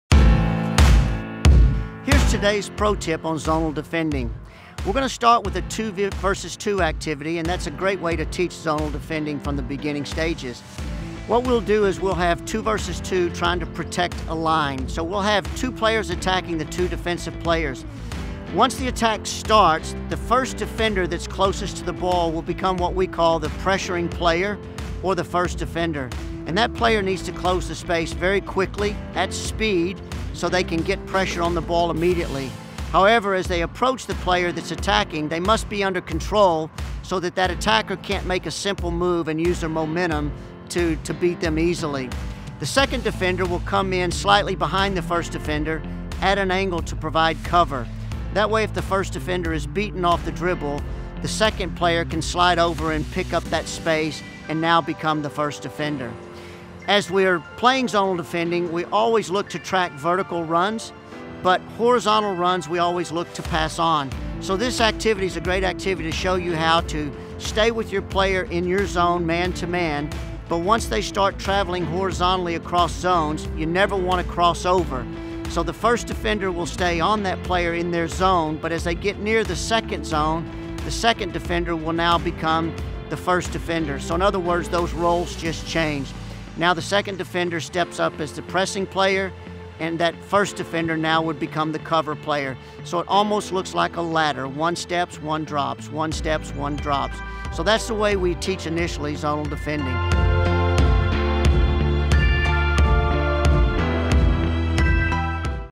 Soccer Tips: Fundamentals of Zonal Defending
Zonal defending is a type of defense where defenders are assigned a position rather than a player. This Pro Tip will give you a basic intro to zone defense.

Division I Women’s Head Soccer Coach Randy Waldrum shares his tips on how to teach zonal defending from the beginning stages. This involves a two-on-two activity with two attackers and two defenders. Let’s look at the roles of the defenders.